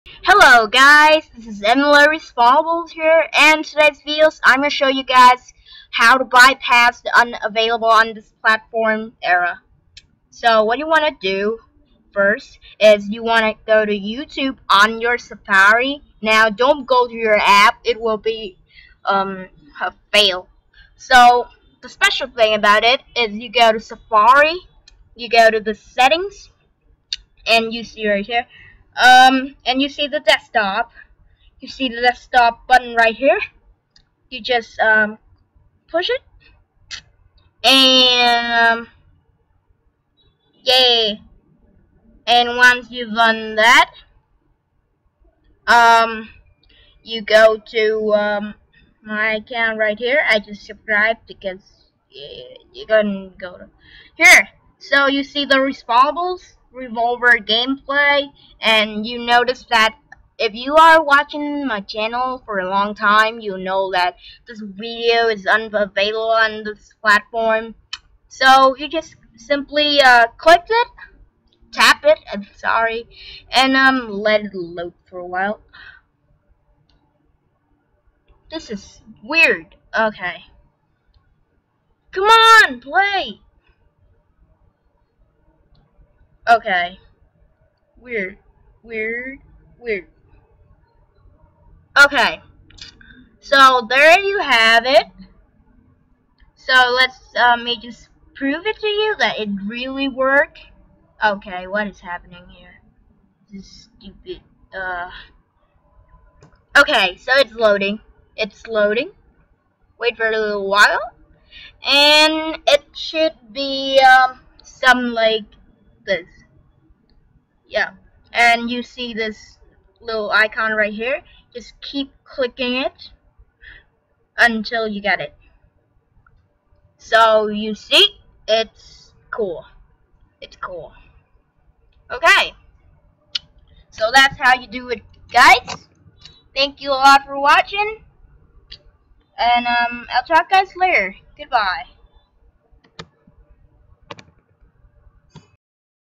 How to by pass the unavailable on this platform error
I've been given lots of comments about this. So here i am showing you how to fix it!.Here's my Facebook